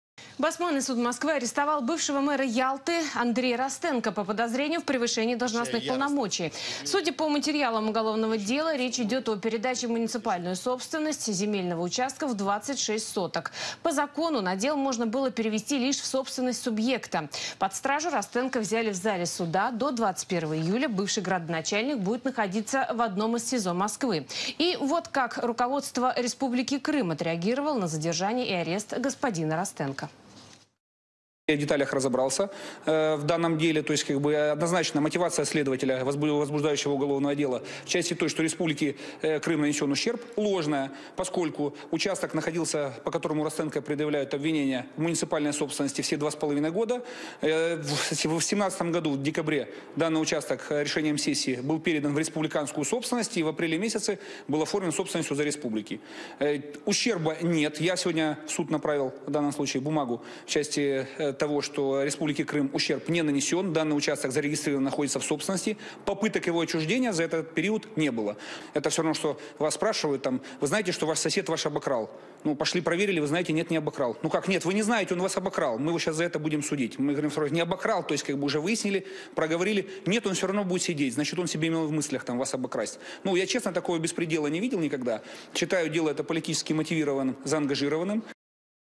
Сергей Аксенов: дело мэра Ялты - политический беспредел - Вести 24
Басманный суд столицы арестовал бывшего мэра Ялты Андрея Ростенко, обвиняемого в превышении должностных полномочий. Срок ареста судья Наталия Дударь установила до 21 июля. Адвокат Ростенко Юрий Авдеев намерен обжаловать решение об аресте его подзащитного в Мосгорсуде.